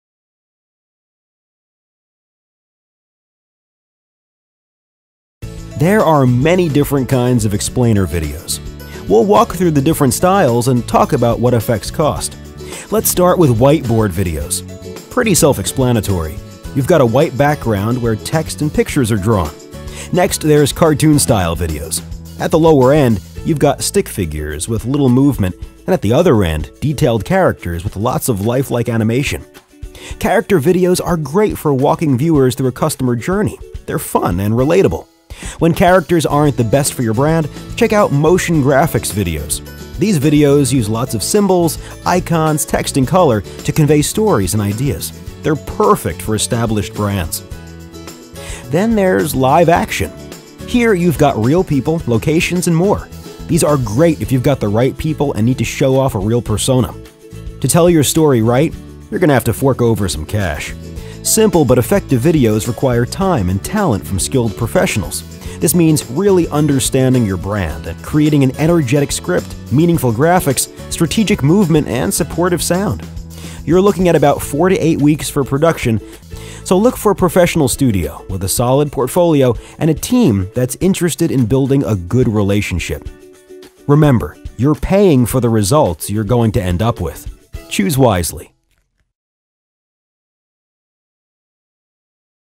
Types of Videos | Filmotion
A video that explains the types of videos for your brand/business